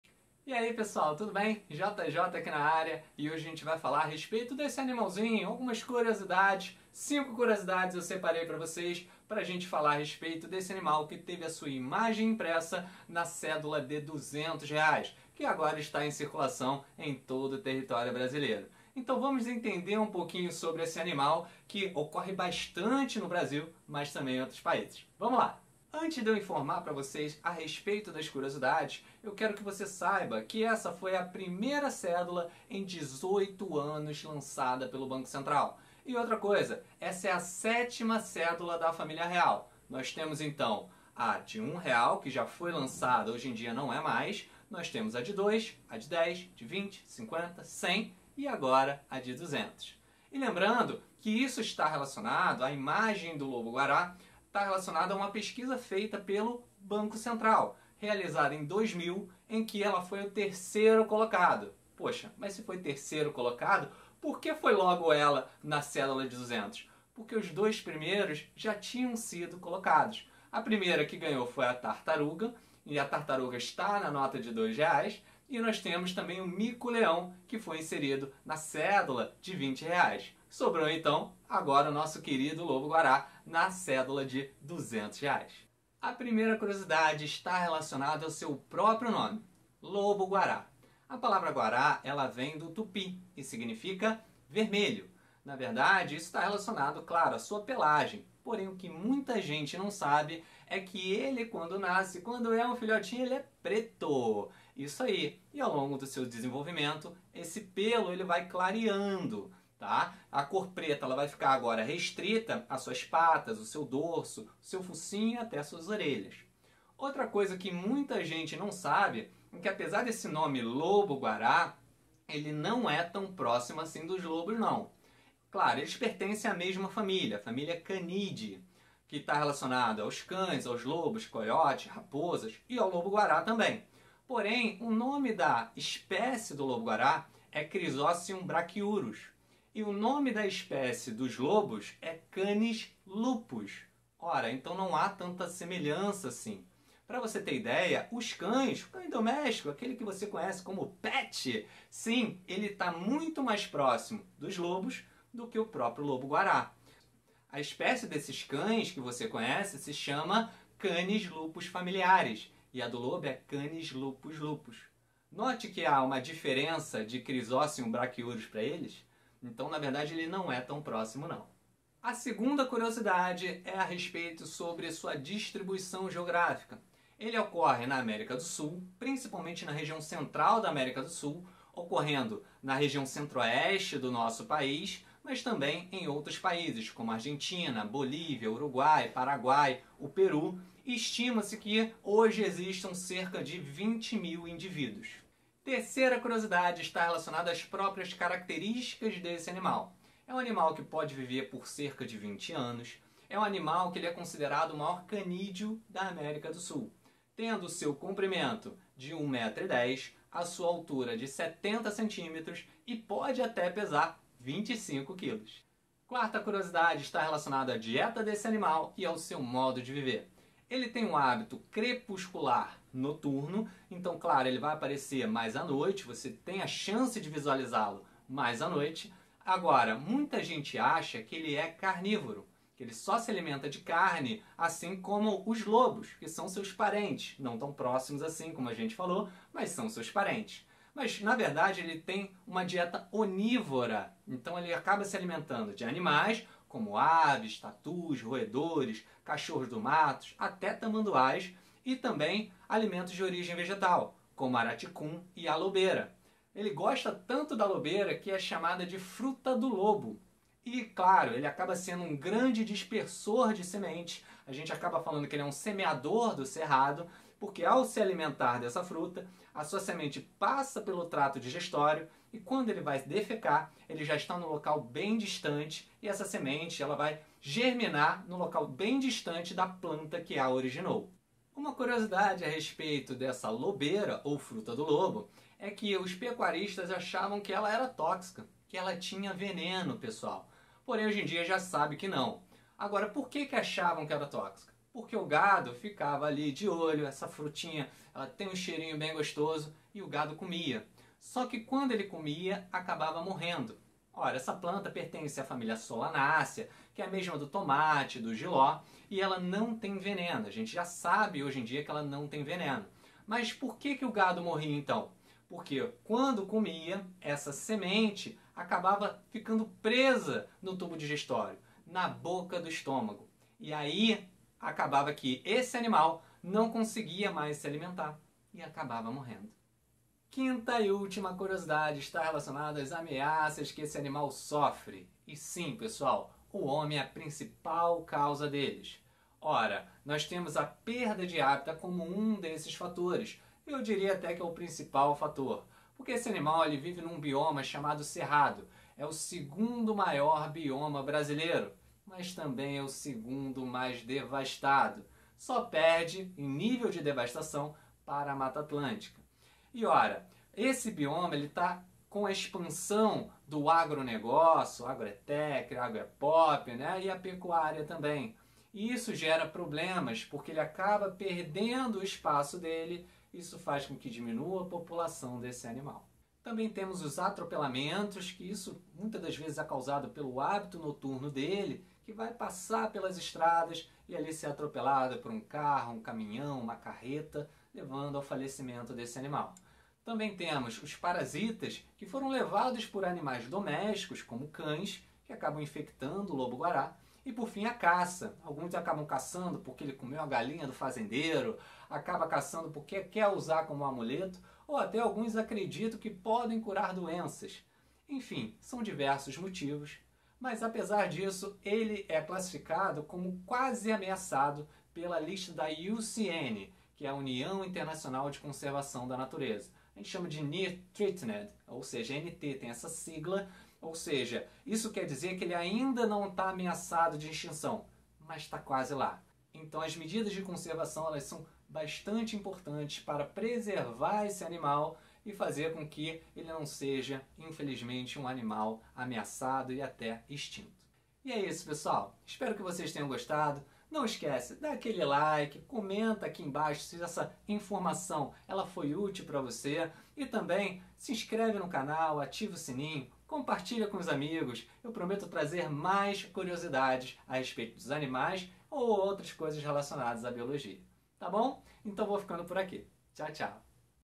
5 CURIOSIDADES QUE VOCÊ NÃO SABIA SOBRE O LOBO-GUARÁ
No vídeo de hoje vamos falar sobre 5 curiosidades do lobo-guará, este belo animal que está com a sua imagem estampada na cédula de 200 reais.